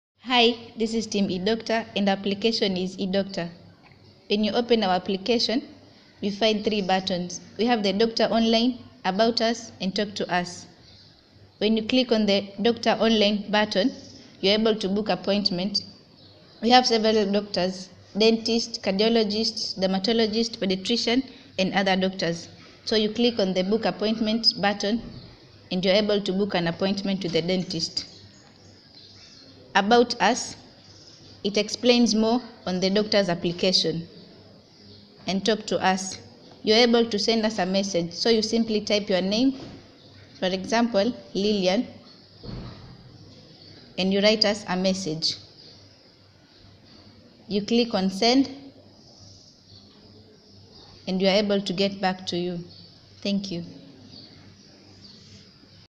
E-doctor Mobile application Technovation 2017
We E-doctor team from Qubaa Muslim school with our application E-doctor